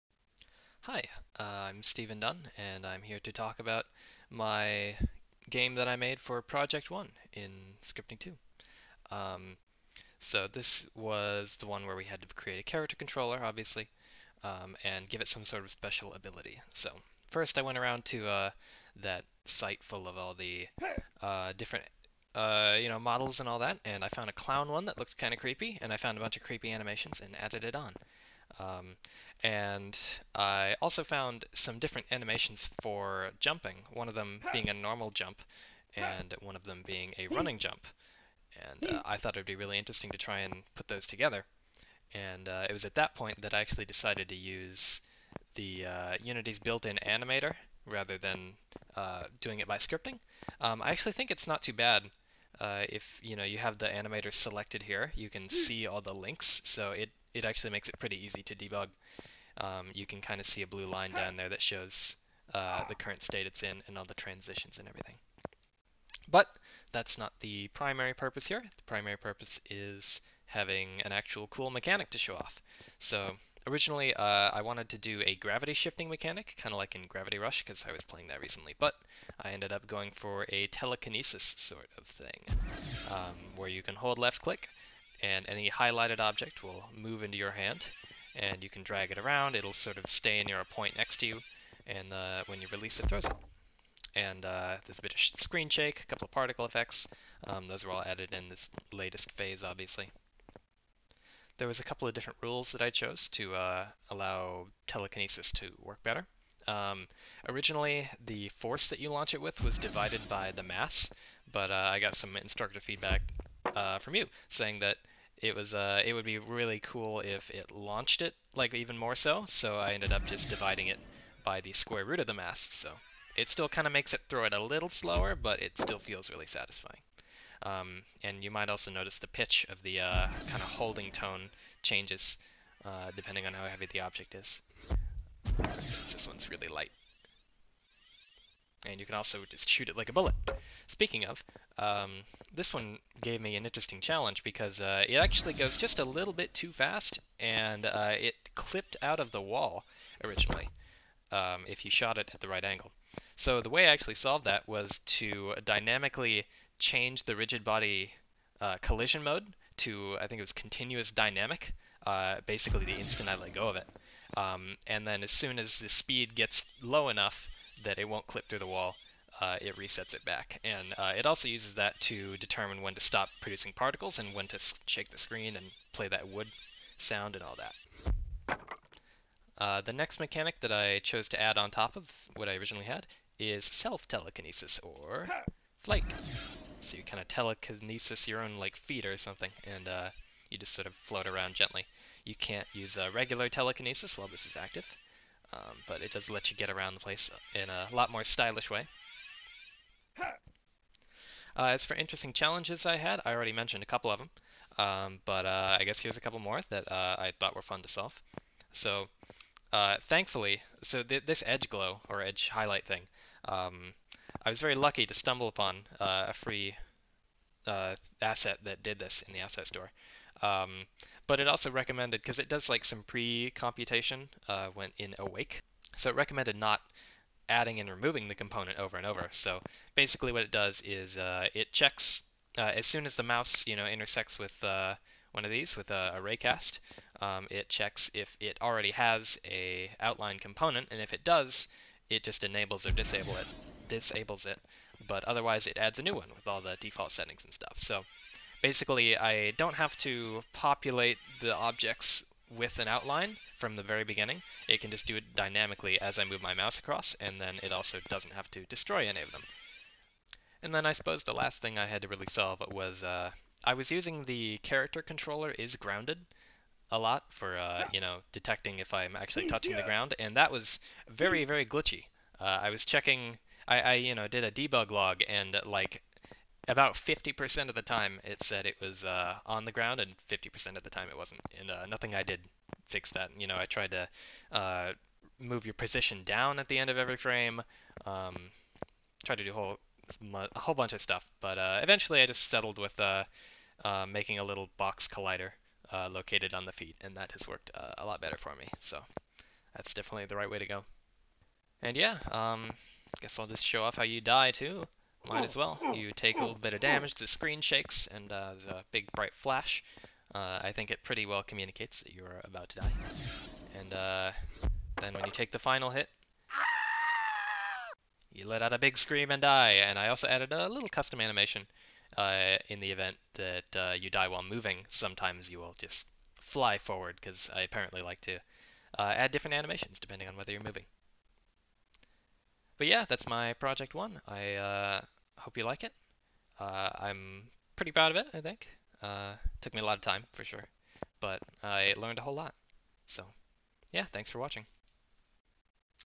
Scripting II: Telekinesis Mechanic Presentation
A more in-depth video about the game I made for Scripting II in Fall 2020. It covers how I created the animations and abilities, and I also talk about the most interesting problems I had to solve while developing the game.